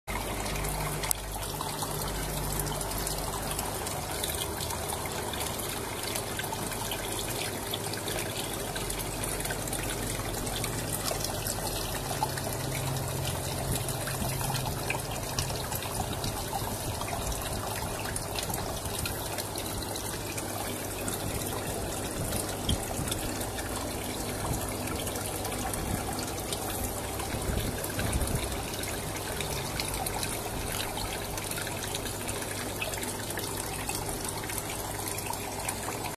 Maxwell Koi Ur0911-1005.MP4
Good bye my dear Ur0911. Spooked by power failure and committed suicide by jumping out of the pond. Sep.2009 to Sep.2010. Miss you a lot.